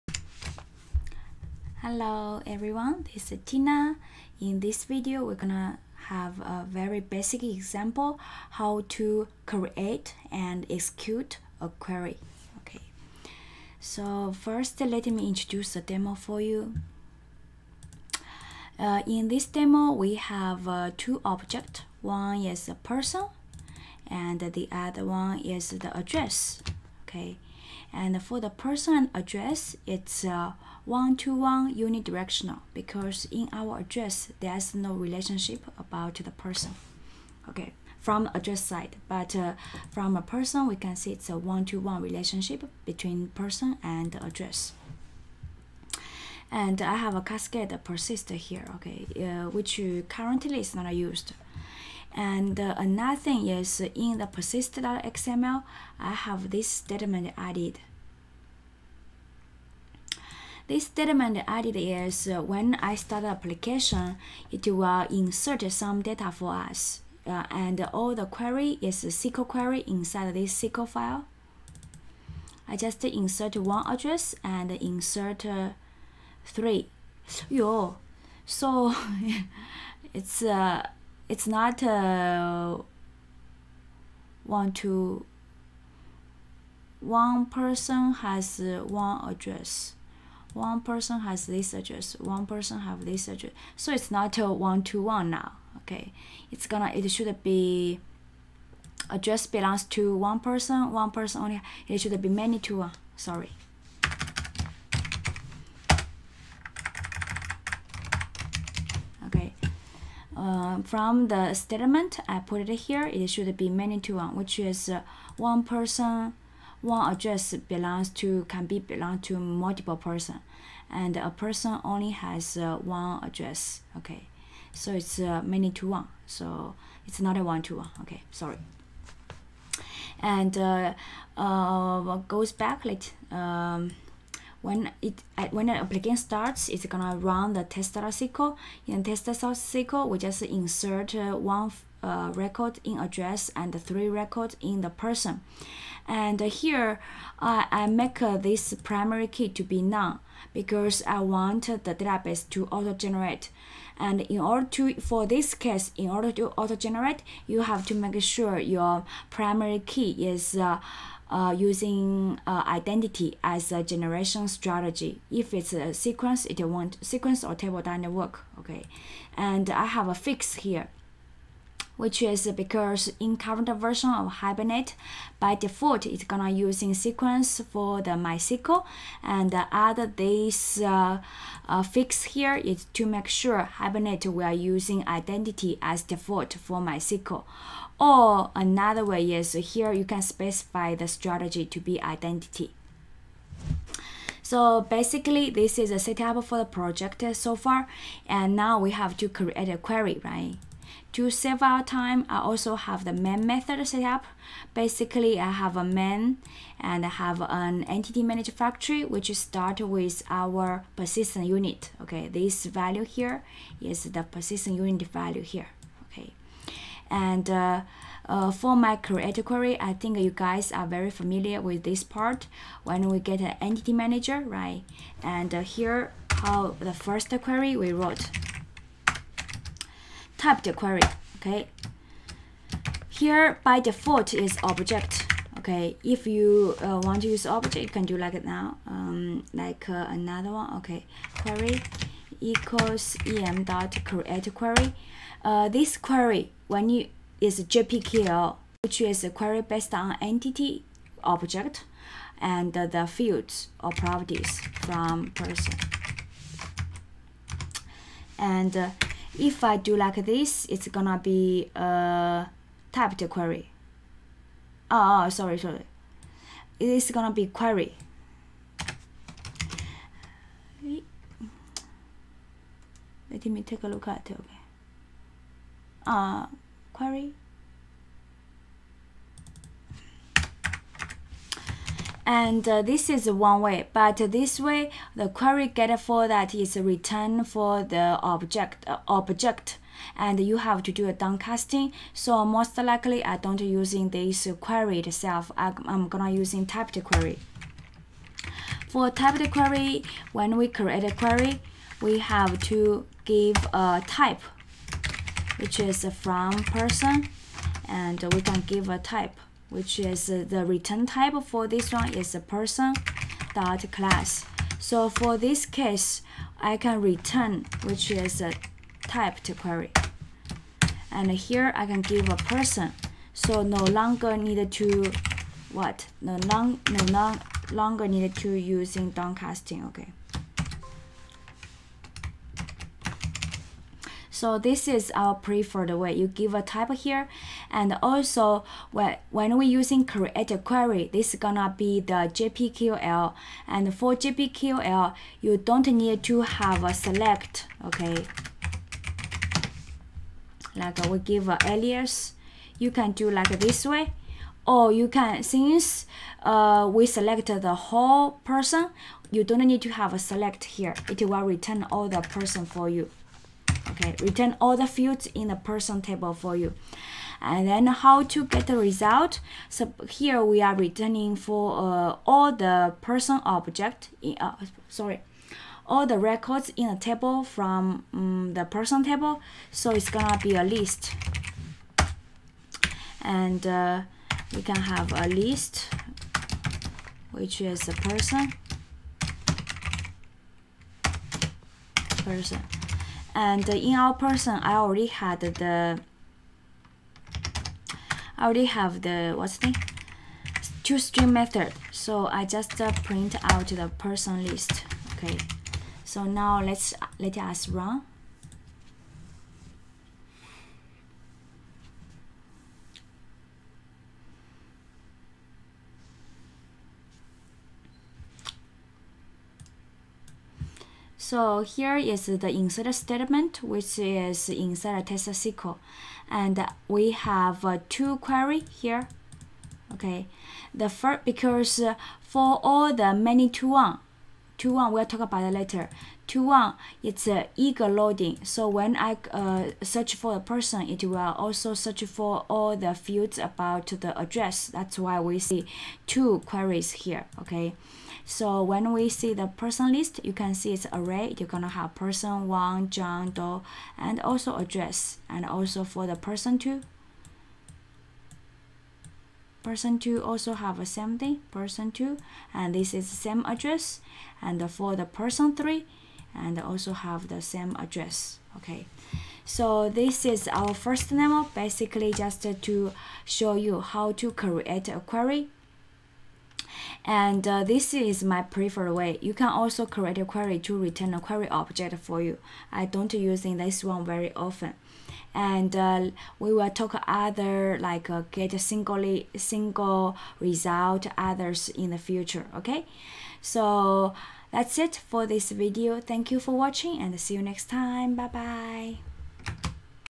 JPA Queries - Creating and Executing
In this video, shows you how to create and execute a query.